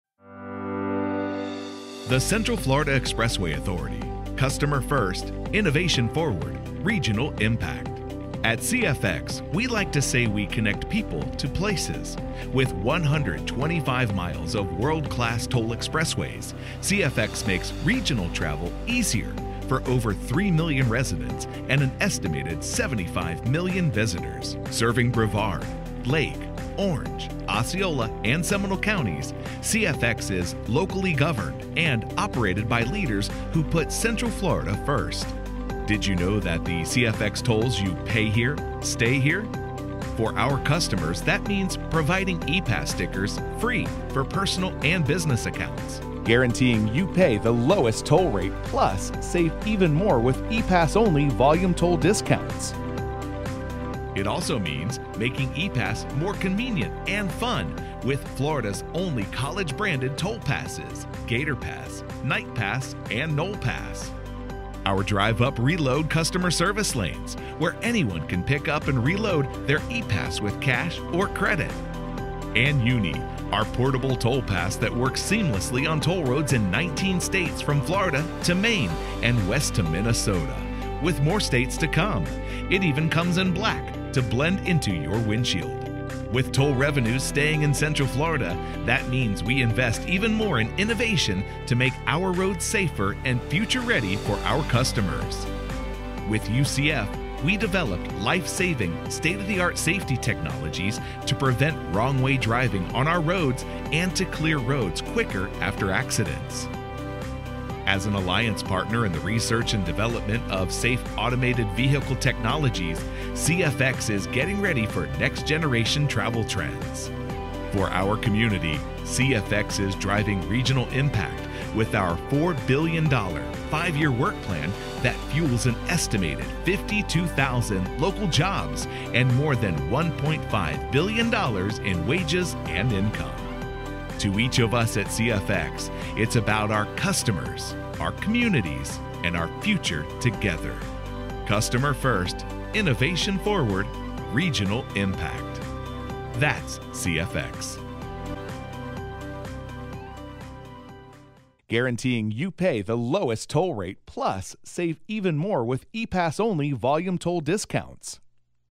CFX - Customer First. Innovation Forward.
The Central Florida Expressway Authority (CFX) is an independent agency of the state that operates and maintains a regional network of expressways for over 3 million residents and an estimated 75 million visitors in Brevard, Lake, Orange, Osceola and Seminole Counties.